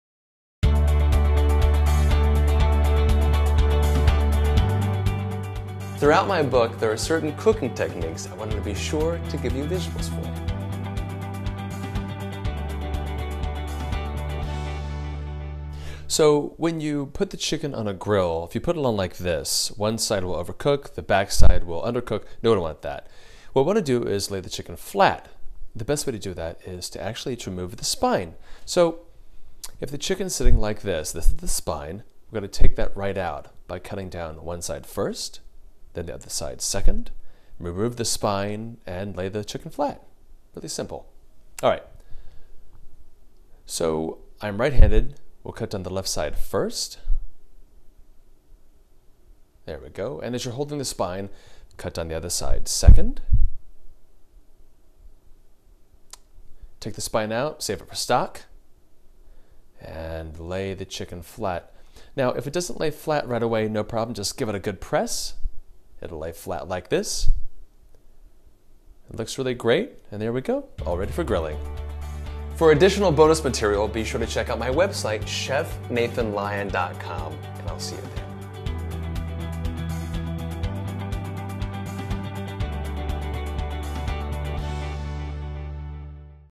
Great Food Starts Fresh - PAGE 224 - Grilled Whole Chicken
Chef Nathan Lyon shows you how to remove the spine from the chicken in order to cook his Grilled Whole Chicken recipe from Great Food Starts Fresh, page 224.